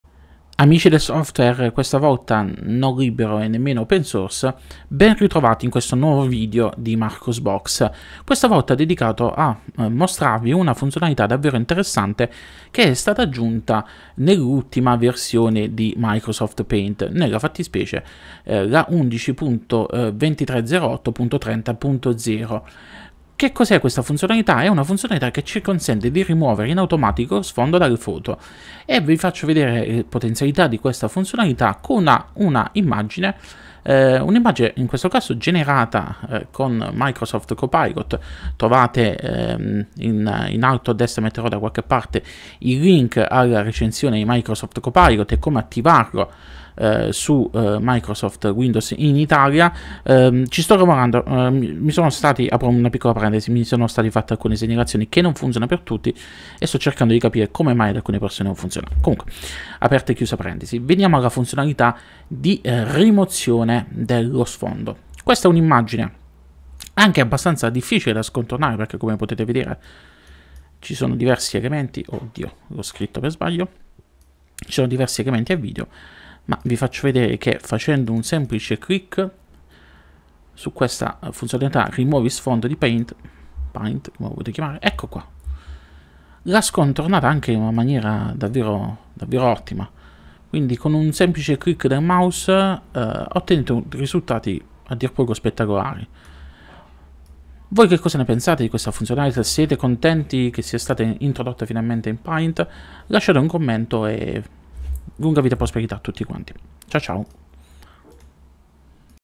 Microsoft Paint: come rimuovere lo sfondo con un clic
Sull'ultima versione di Microsoft Paint per Windows 10/11 è stata aggiunta la funzionalità di rimozione automatica dello sfondo.
Ecco come funziona.